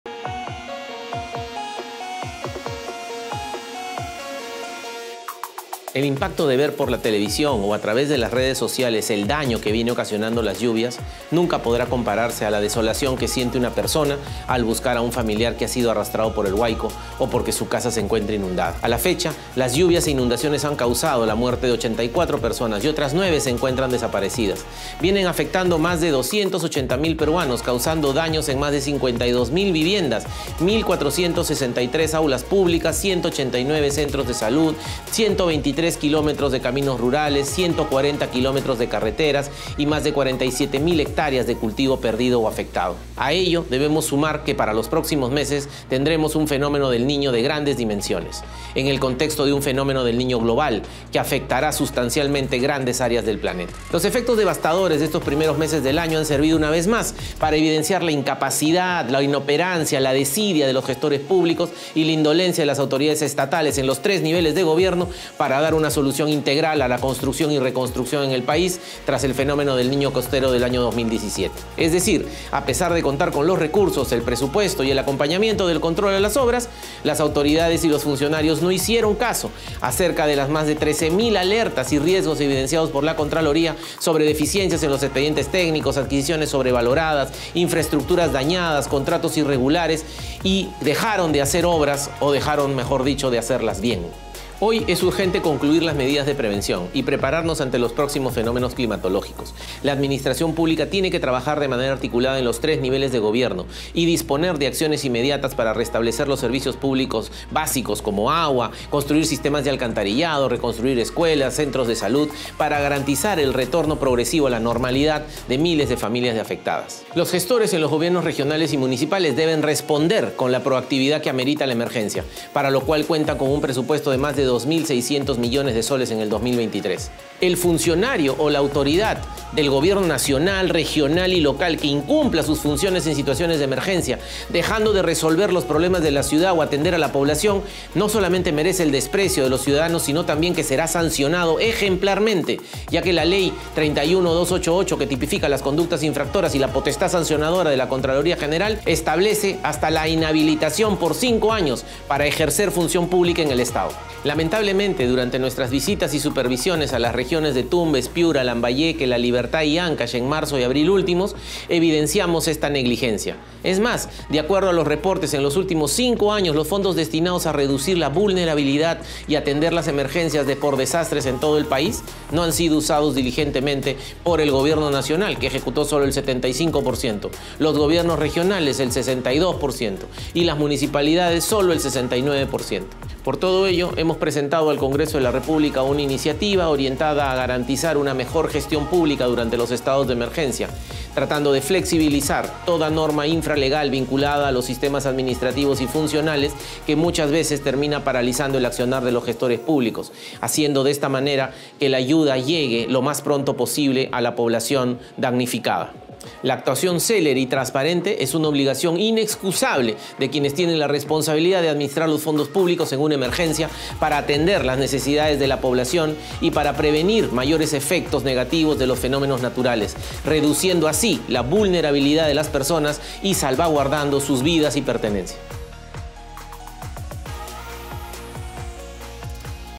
Puntos de vista | Acción o inhabilitación: Cuando los funcionarios no responden a la emergencia
El contralor Nelson Shack sostuvo que el funcionario o autoridad que incumpla sus funciones en situaciones de emergencia, y deje de resolver los problemas de la ciudad o de atender a la población será sancionado ejemplarmente, ya que la Ley 31288, establece hasta la inhabilitación para ejercer en el Estado.